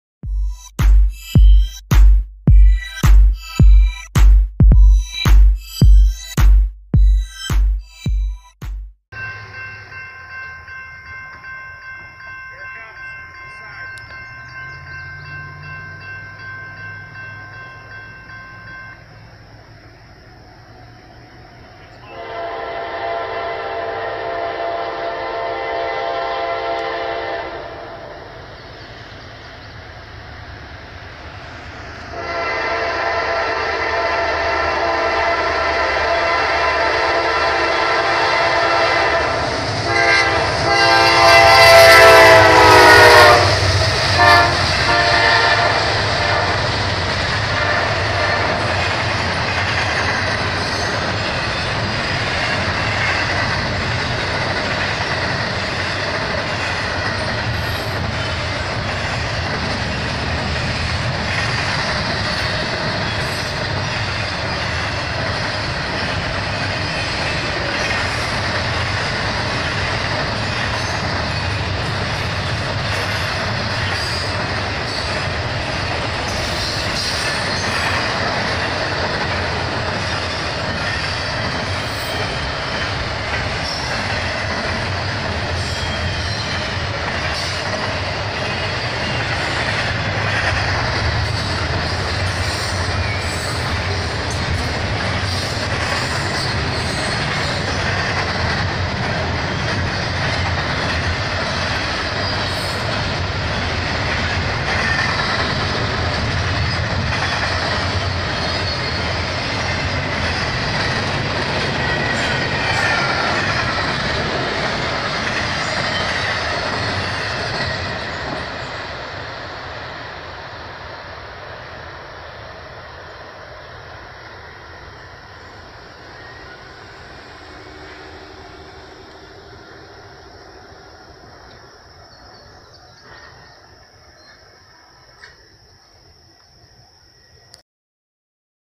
VRE & CSX Speed through Virginia! Part 2
Now, after the VRE Passenger Train passes, a CSX Intermodal Q156 Train goes by at 65 MPH with a BNSF Dash 9 Locomotive with H2 Paint trailing right behind it!

SATX Railfanning Productions. LLC 2018. Any recordings, duplications, or posting anywhere is prohibited without permission from the owner.